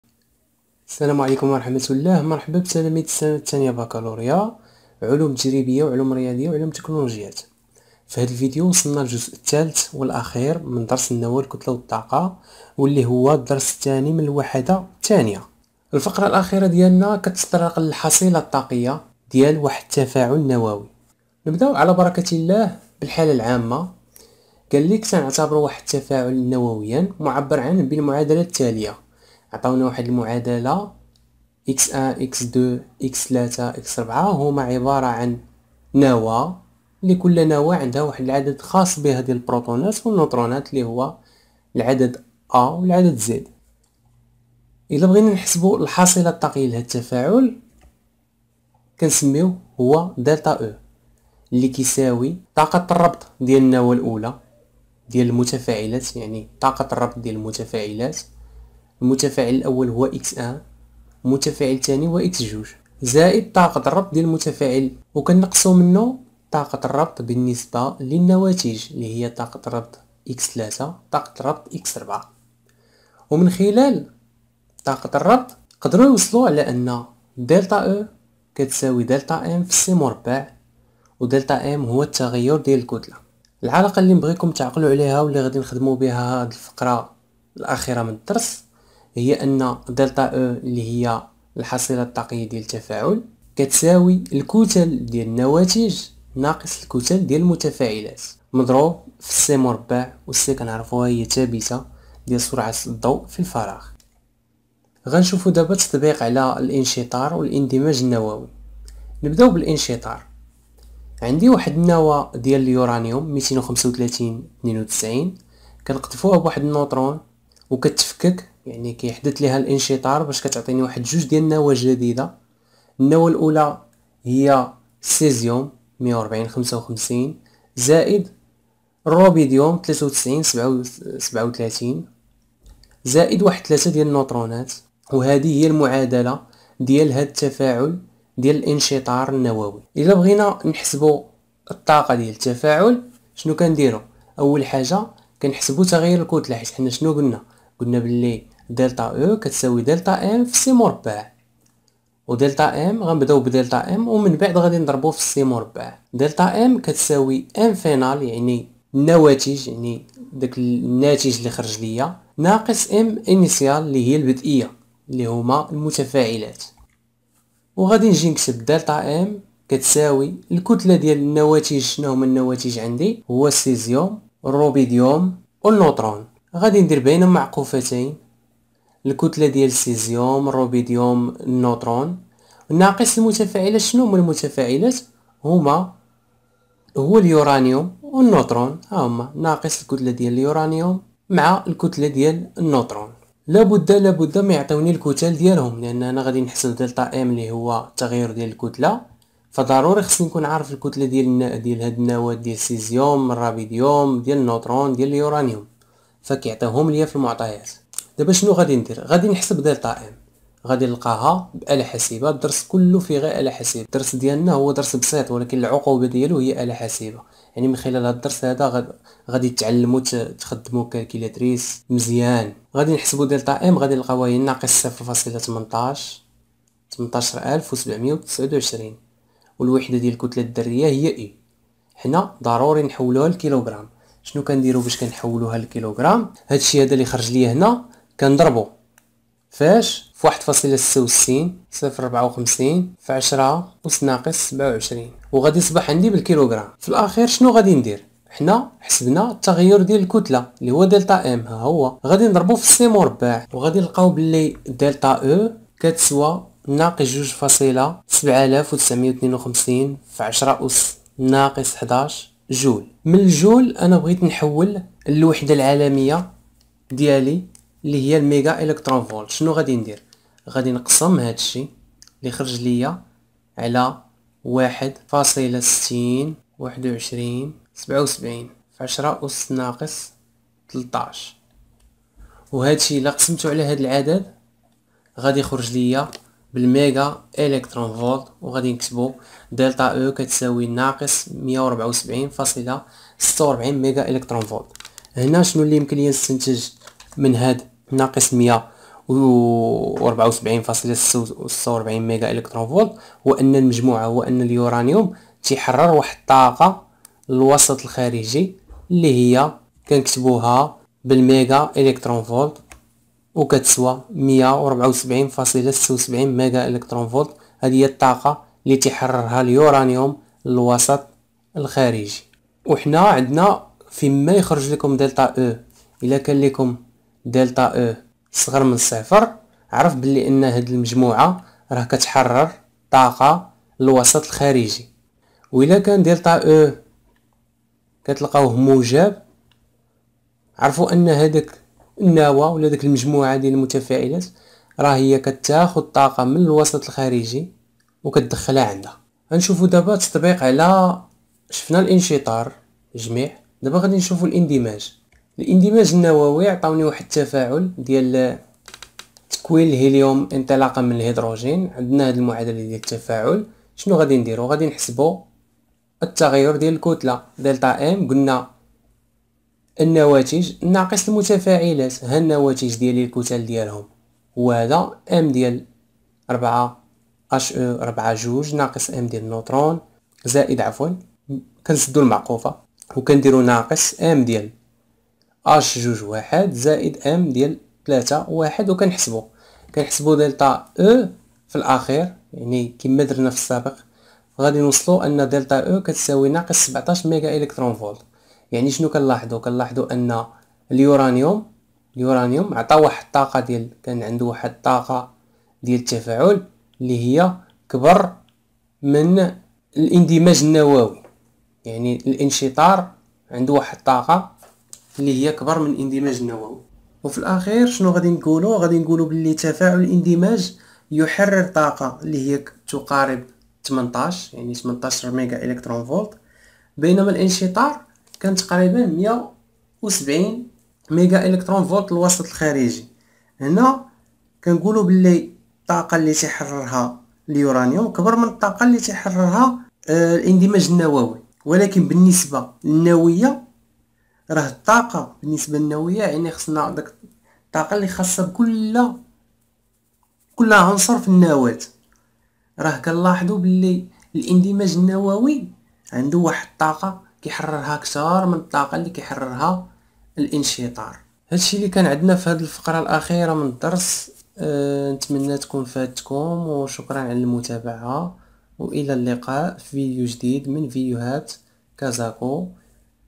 الثانية باك علوم فيزيائية | التحولات النووية درس النوى الكتلة والطاقة الجزء الاخير
هذا الفيديو يخص تلاميذ السنة ثانية باكالوريا و يتضمن شرح التحولات النووية درس النوى الكتلة والطاقة الجزء الاخير